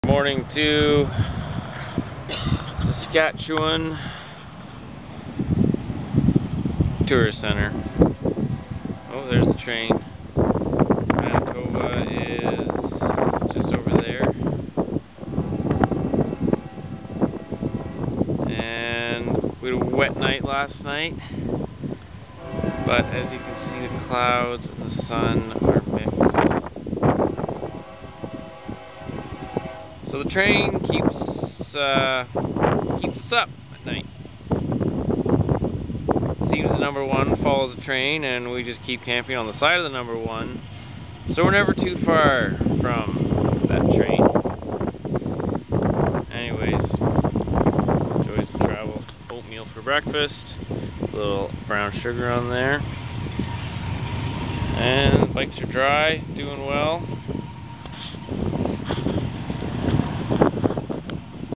Biking Across Canada - SK/Manitoba Border - Morning Oatmeal
Camped behind the tourist information stop just before entering Manitoba, a massive rain storm leaving a full inch of water in our camp mugs hit us overnight!